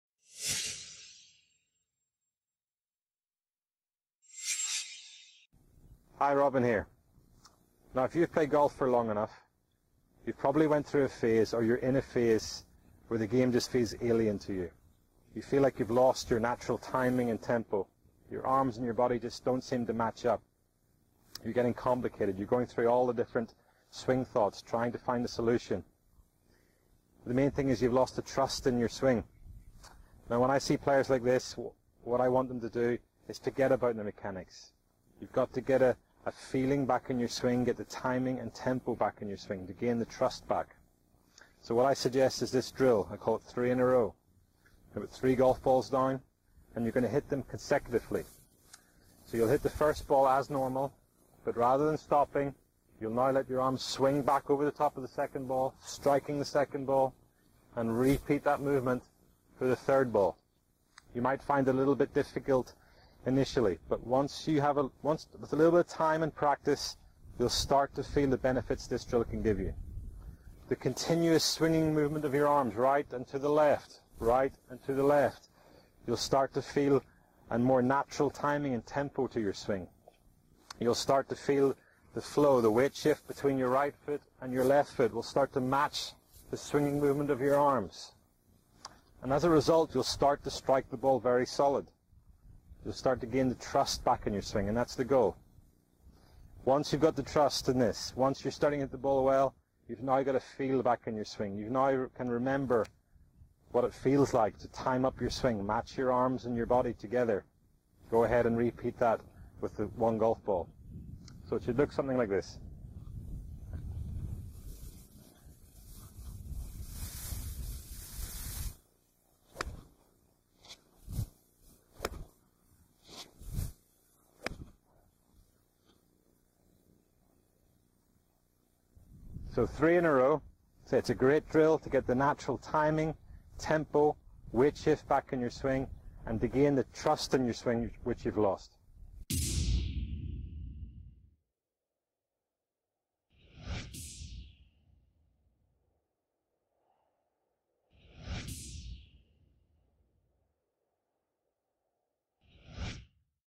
In A Slump? - 3 In a Row Drill
LOST YOUR GOLF GAME- PGA Professional RObin Symes shows his favorite drills to help players get the feeling back in their swing, and get out of the slump they are in.

Transcript
Hi, Robin here. Now if you've played golf for long enough, you've probably went through a phase or you're in a phase where the game just feels alien to you. You feel like you've lost your natural timing and tempo. You're arms and your body just don't seem to match up. You're getting complicated. You're going through all the different swing thoughts, trying to find the solution. The main thing is, you've lost the trust in your swing. Now when I see players like this, what I want them to do is to get about the mechanics. You've got to get a feeling back in your swing, get the timing and tempo back in your swing to gain the trust back. So what I suggest is this drill. I call it "3 in a row."
You put three golf balls down, and you're going to hit them consecutively. So you'll hit the first ball as normal, but rather than stopping, you will now let your arm swing back over the top of the second ball, striking the second ball, and repeat that movement for the third ball. You might find it a little bit difficult initially, but with a little bit of time and practice, you'll start to feel the benefits this drill can give you. The continuous swinging movement of your arms, right and to the left, right and to the left. You'll start to feel a more natural timing and tempo to your swing. You'll start to feel the flow of the weight shift between your right foot and your left foot will start to match the swinging movement of your arms. And as a result, you'll start to strike the ball very solid. You'll start to gain the trust back in your swing, and that's the goal. Once you've got the trust in this, once you've started to hit the ball well, you've now got a feel back in your swing. You now can remember what it feels like to time up your swing, match your arms and your body together. Go ahead and repeat that with one golf ball. So it should look something like this.
So three in a row. It's a great drill to get the natural timing, tempo, weight shift back in your swing, and to gain the trust in your swing, which you've lost.